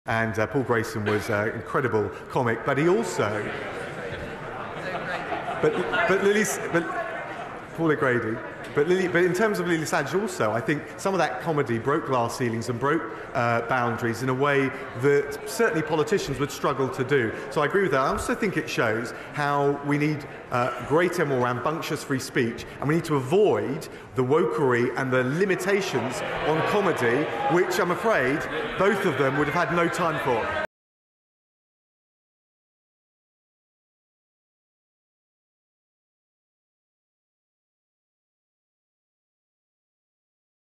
Dominic Raab mistakes late Paul O'Grady for Paul Grayson
Dominic Raab mistakes late Paul O'Grady for Paul Grayson | Dominic Raab calls Paul O'Grady 'Paul Grayson' during House of Commons tribute. The Deputy Prime Minister laughed cluelessly as MPs reacted to the gaffe, before correcting himself.